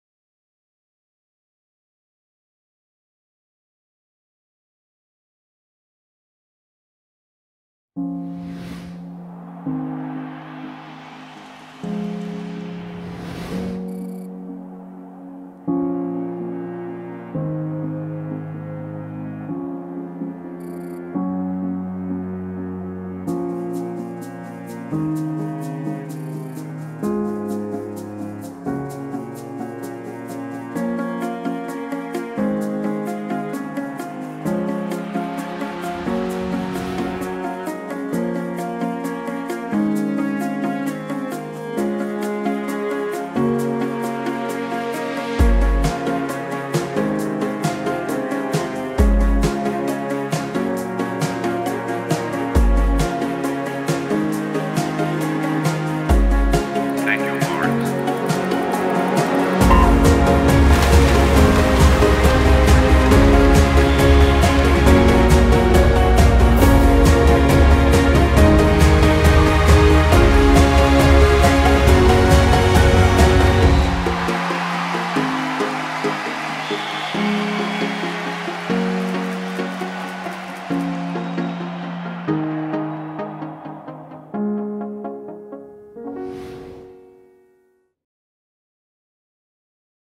OMEGA Cronometrando los 100 metros libres (subtitulado en español)
El vídeo oficial de OMEGA en el que explica cómo se cronometran los tiempos en natación. He añadido subtítulos en español.